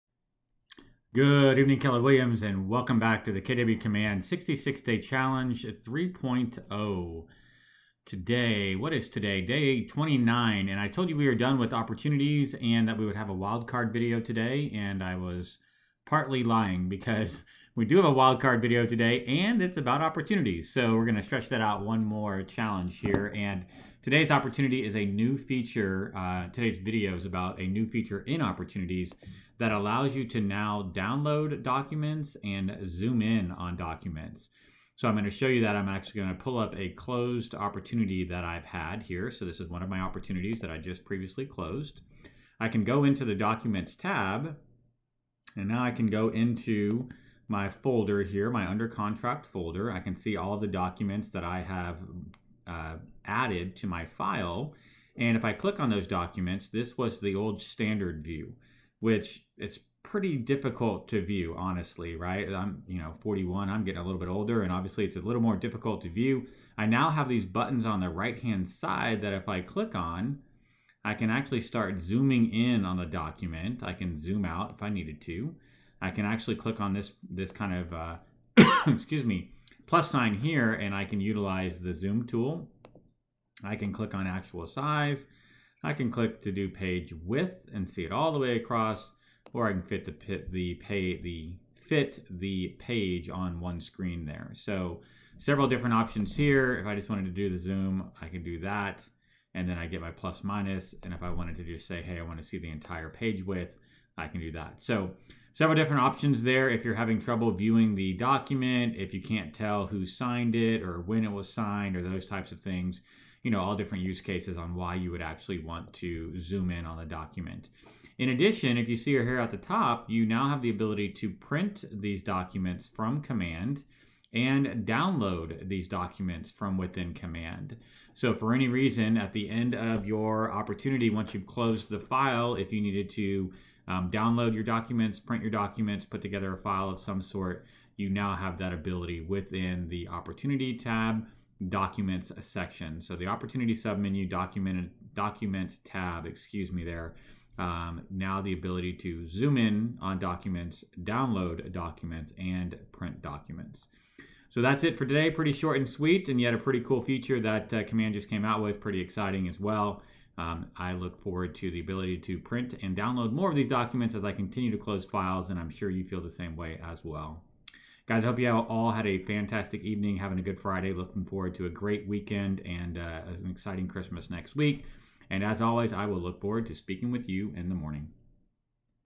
Zoom, Print and Download Documents in Opportunities | KW Command 66 Day Challenge 3.0 Day 29
When Documents are viewed from within the Documents tab of the Opportunities submenu of the KW Command platform, they can be somewhat difficult to read because of the size of the document on the screen.  Well, that's no longer an issue.  KW Agents now have the ability to Zoom In on their documents, size them to fit the entire screen or fit the width of the screen as well.  Additionally, KW Agents can now Download and Print their documents straight from the Documents tab within KW Command Opportunities.  Watch this short video to learn how to do all of the above.